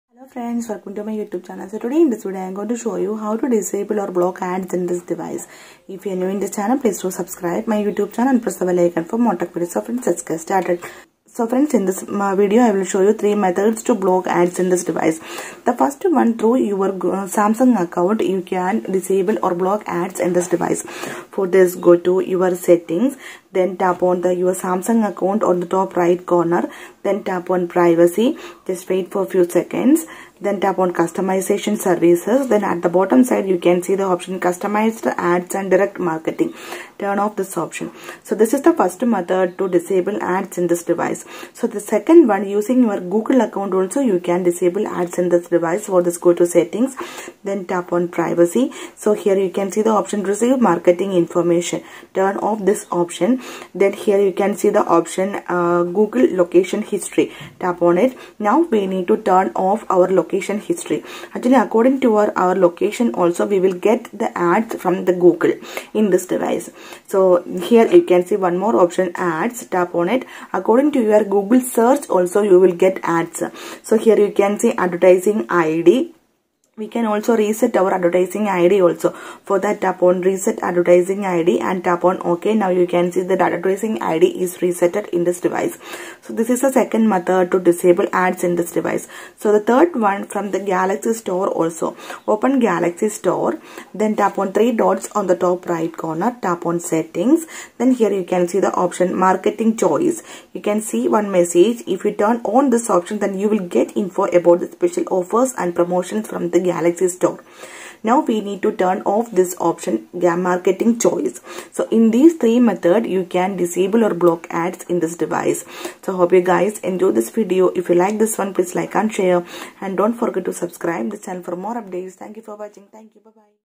How To Disable Ads in New Samsung Galaxy A12
Hi friends this video shows you that " How to disable ads in Samsung galaxy A12 "